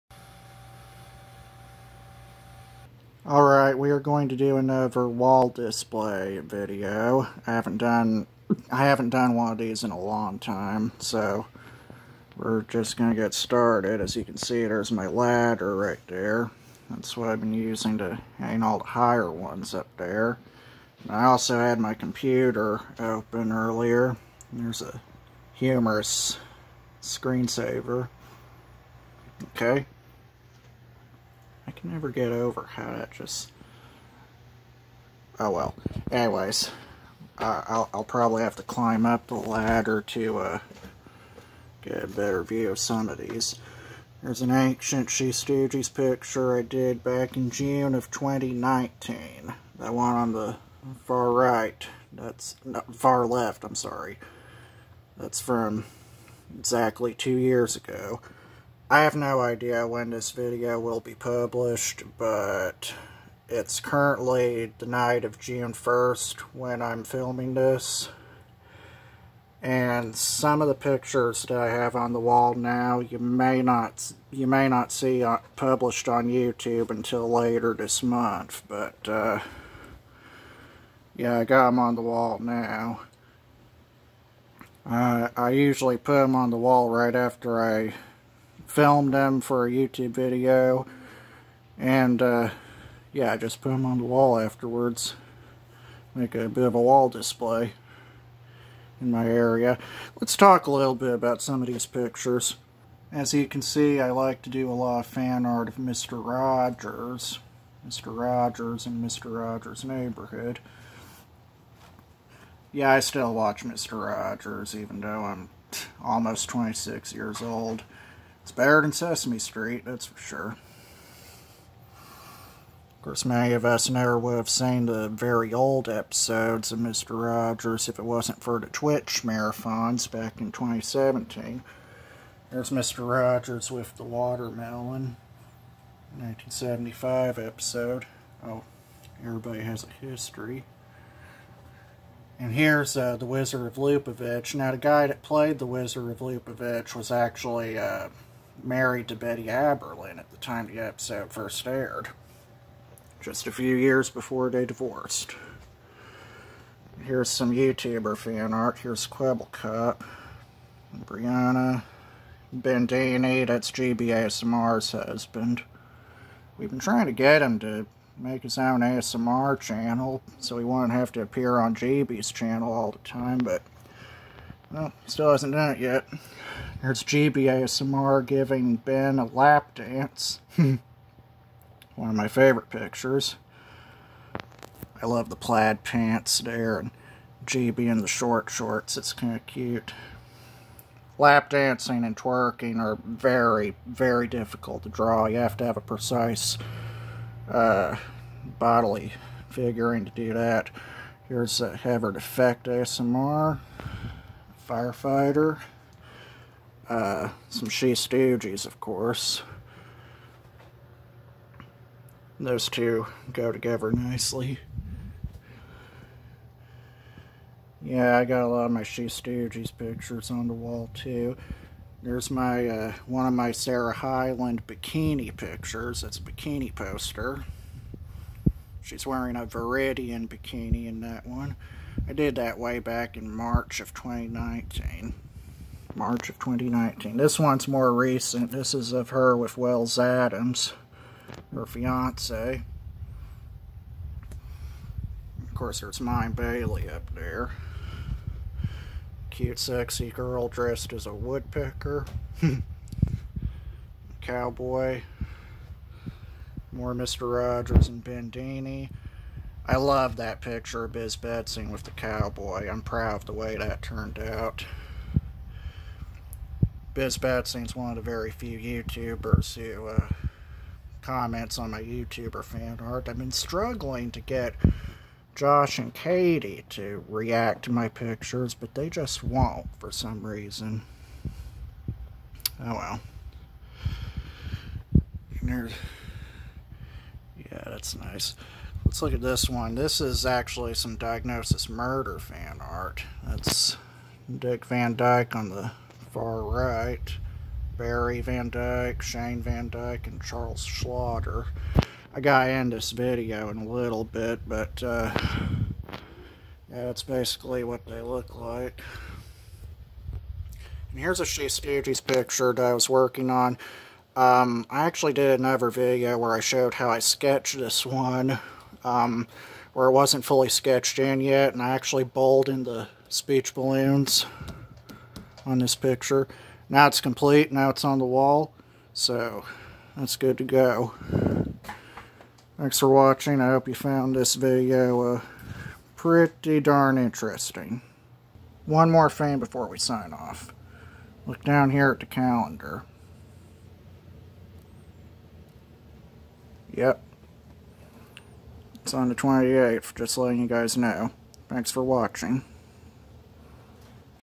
Yet another Wall Display for June 2, 2021 (recorded on June 1, 2021)
This is yet another wall display vlog, showing off my bizarre experimental and fan-art in all it's glory.

Also, much later this month it'll be my 26th birthday :), so please remember that this month.

Category: Art